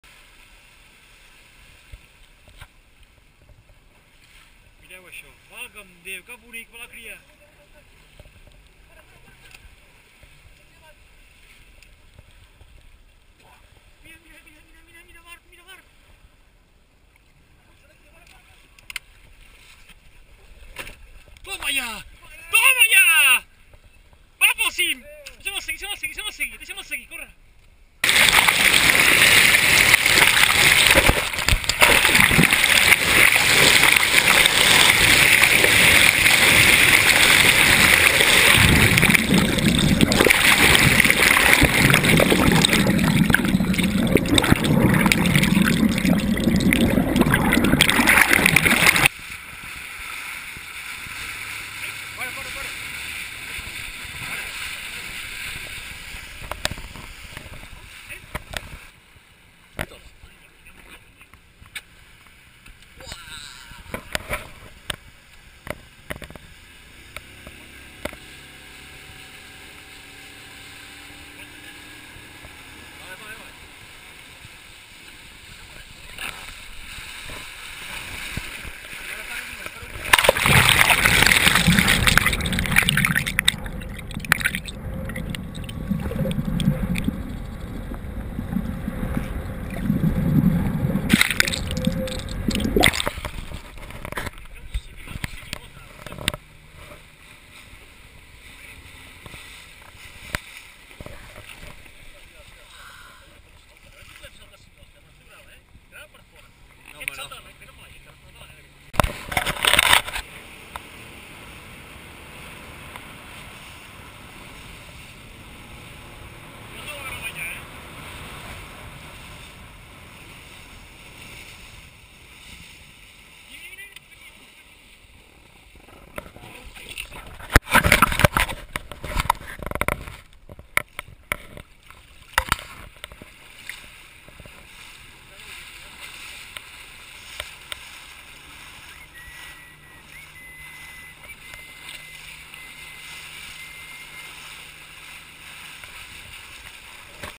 Dofins Octubre 2012
Dofins entre Llançà i Port de la Selva tornant de Cap de Creus. Diumenge 7 d'octubre de 2012 a les 10:30